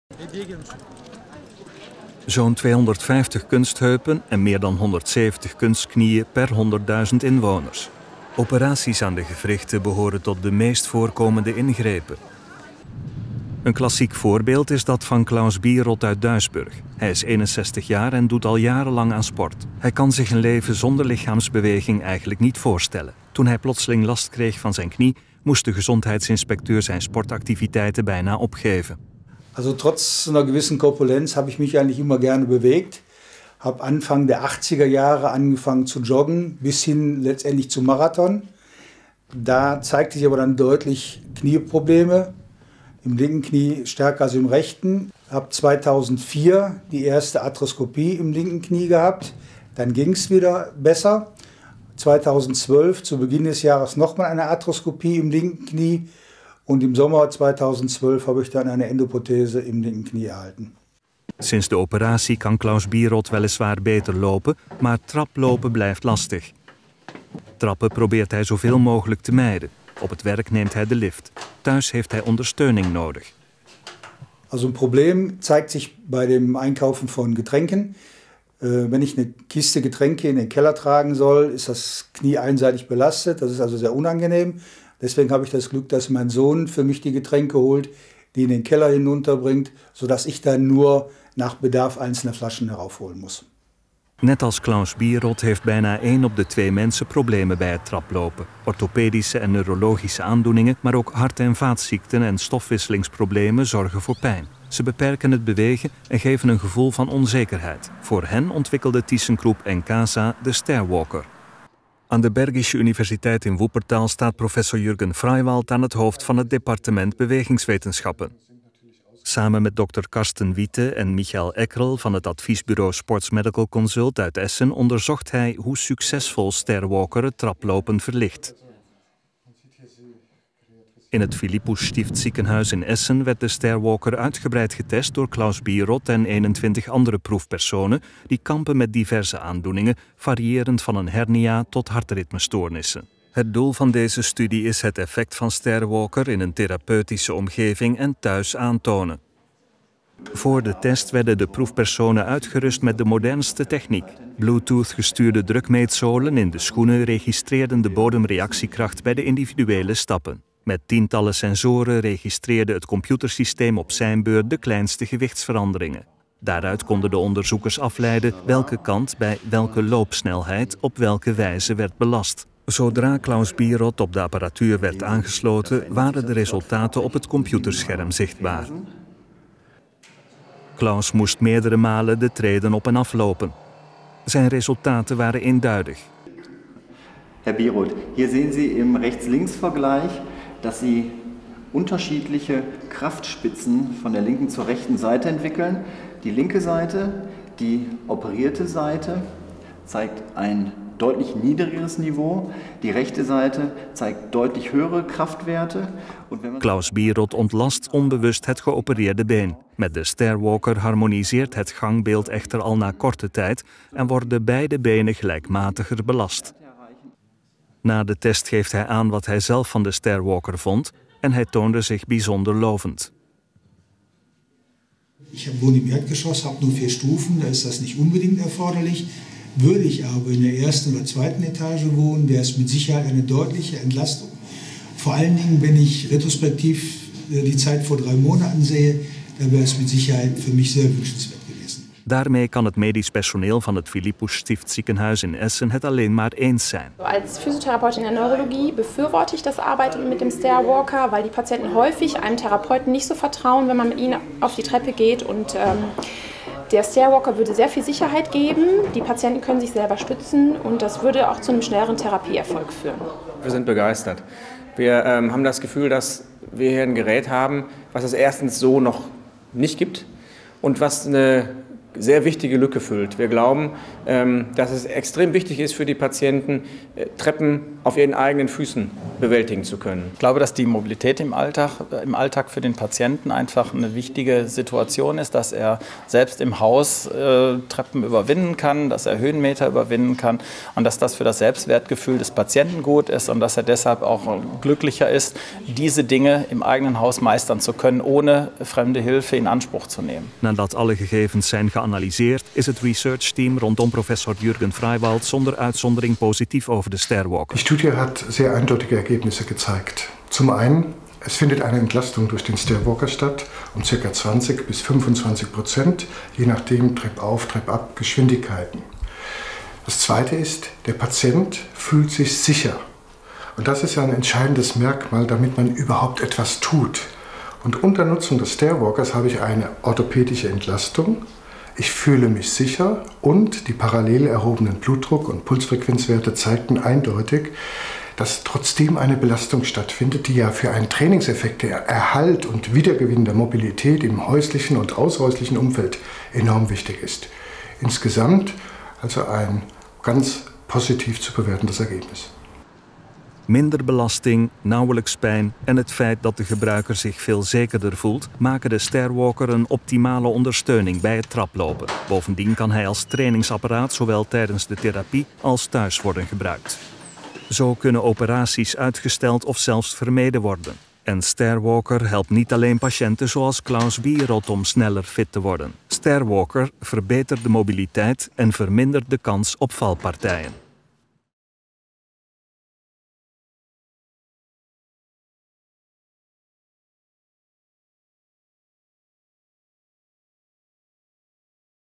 StairWalker ThyssenKrupp Encasa
De StairWalker van ThyssenKrupp Encasa - leverancier van mobiliteitsoplossingen - laat mensen met een orthopedische, neurologische of hart- en stofwisselingsaandoening voor het eerst veilig, zelfstandig en met minder pijn trainen. Dit blijkt uit een studie van de Bergische universiteit Wuppertal in Duitsland. Professor Jürgen Freiwald, hoofd van de faculteit voor bewegingswetenschappen, onderzocht samen met sportwetenschappers van het consultatiebureau SportsMedicalConsult uit Essen de werking van de StairWalker. Deze speciale traplift ondersteunt mensen bij het zelf traplopen.